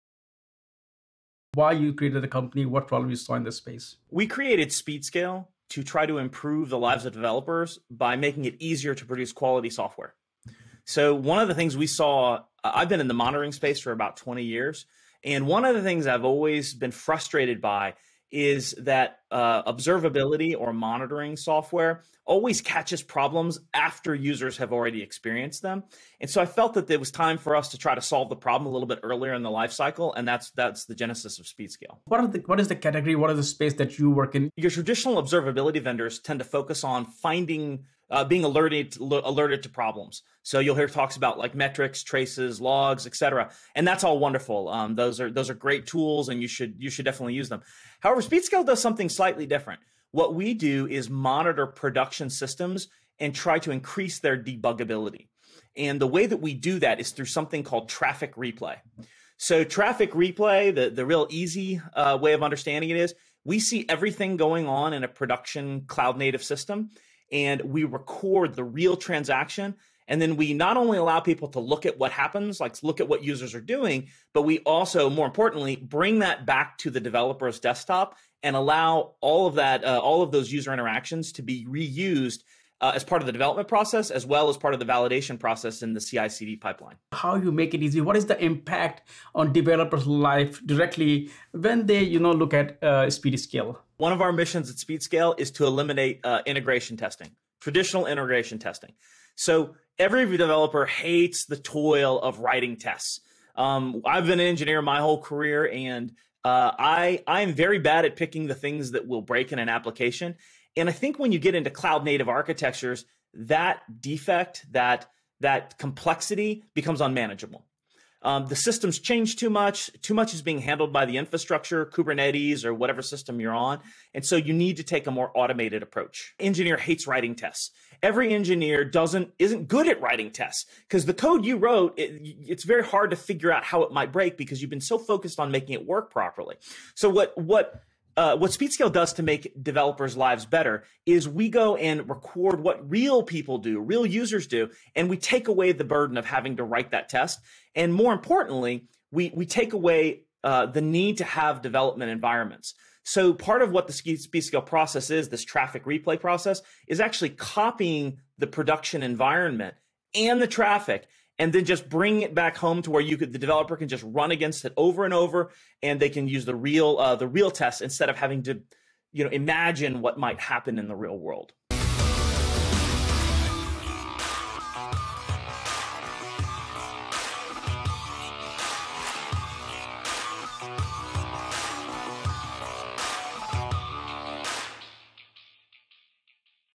Speedscale’s Traffic Replay Helps Developers See And Solve Problems Ahead of Release
Speedscale, the API test automation company, helps developers test new API code under production-like workload conditions. The company was created to make it easier for developers to produce quality software. Matthew LeRay, co-founder and CTO of Speedscale, explains how during his 20 years working in the monitoring space he felt that observability or monitoring software often caught the problems after users had experienced them. Speedscale was created to solve the problems earlier in the lifecycle. LeRay explains how Speedscale’s approach is different from traditional observability vendors and how the company uses Traffic Replay for utilizing monitoring production systems and increasing their debuggability. To find out more about Speedscale’s Traffic Replay, watch this video.